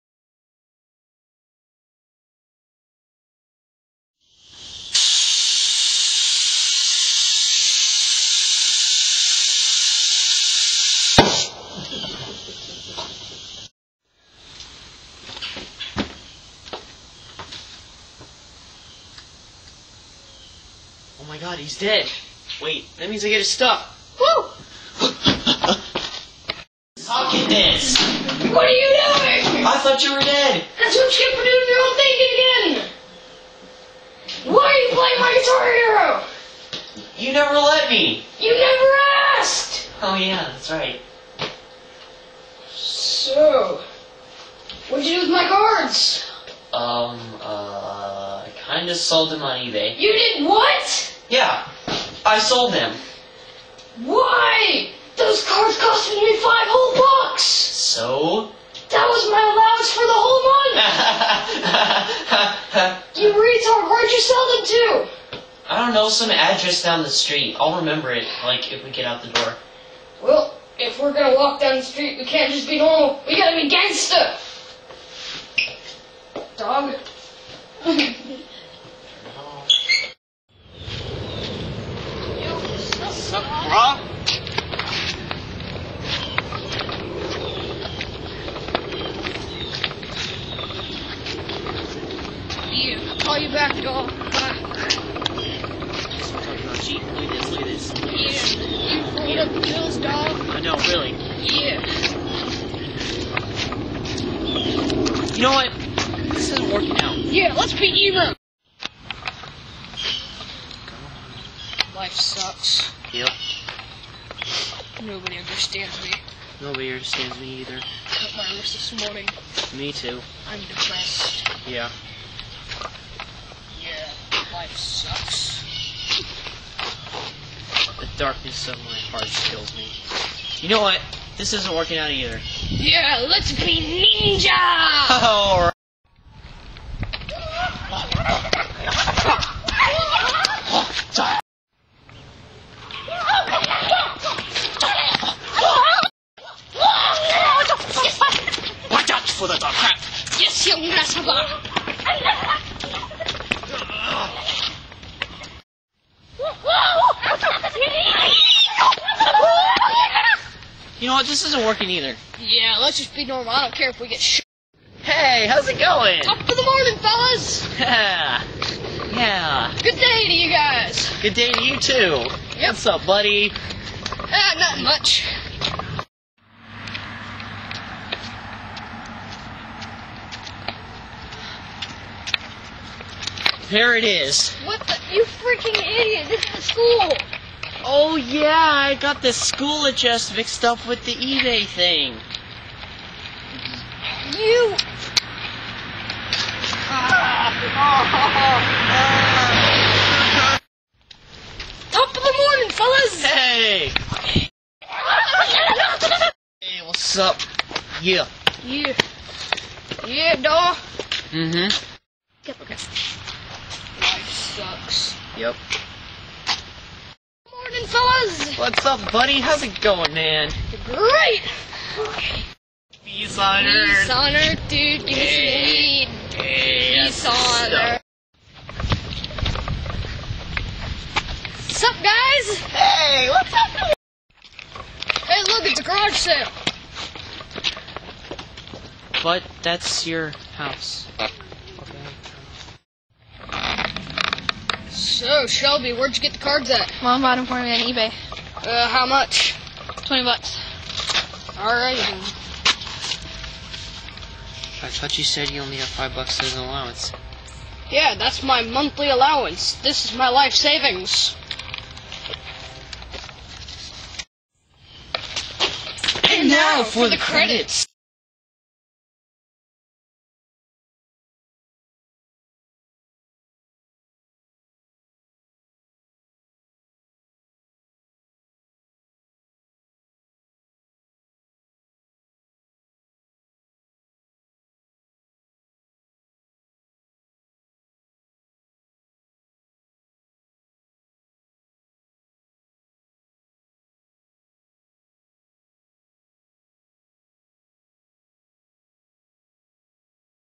Quest for the Deck of Cards
link to the bloopers the storyline is a guy who supposedly dies, his cousin sells his deck of cards on eBay, so they set off to get it back